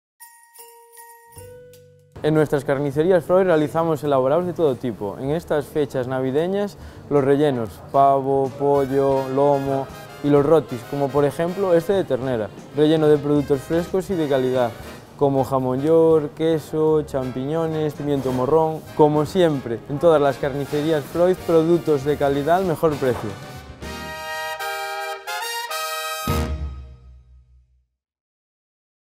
Consejo Froiz: Rellenos de carne de elaboración propia
Los profesionales Froiz de carnicería, como nuestro compañero Andrés, elaboran en estas fiestas navideñas distintos rellenos de carne de pavo, cerdo y ternera con ingredientes de primera calidad. Consulta en tu centro Froiz habitual el amplio surtido disponible.